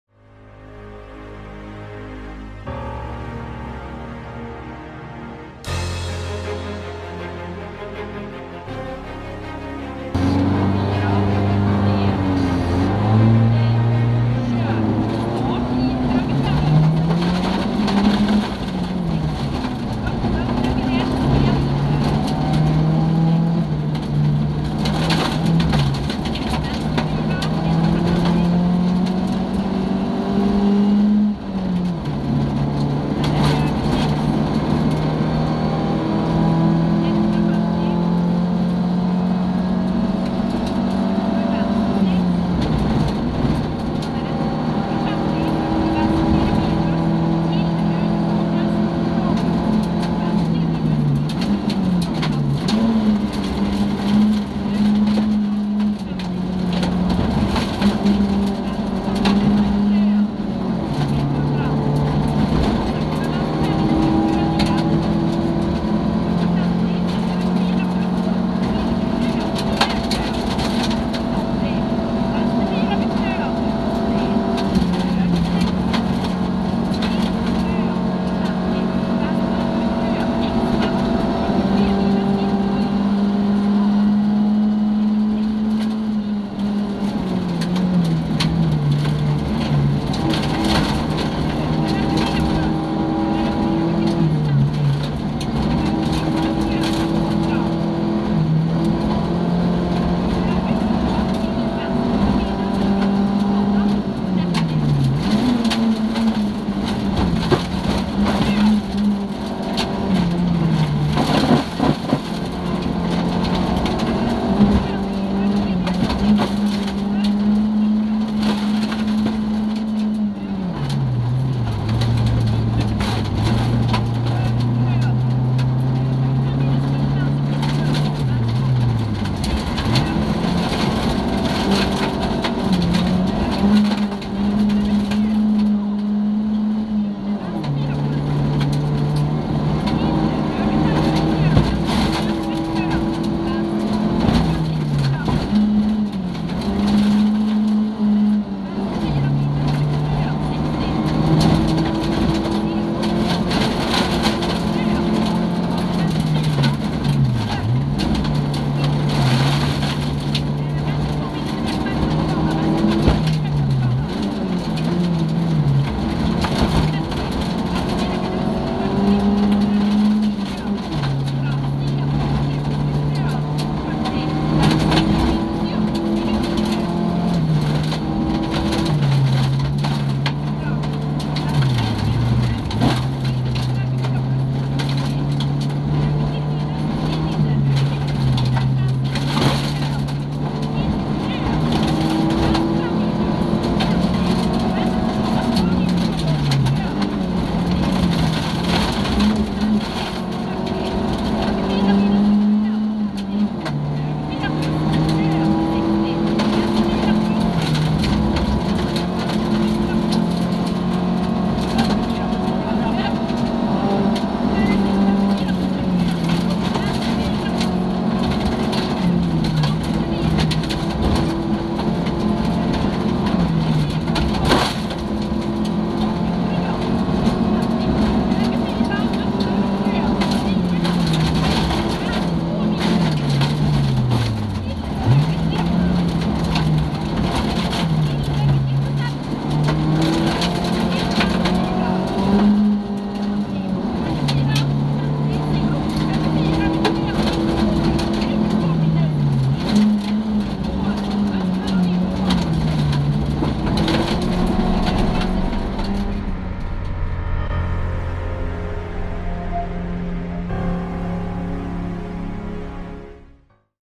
Marknadsnatta 2020 - SS4 - Andreas Magnusson
Incar från Marknadsnatta 2020 SS4

Delad 1:a (C Grupp E)
Delad 2:a (Totalt Grupp E)

Bil: Opel Corsa - Grupp E
Co-Driver: Ella Ramström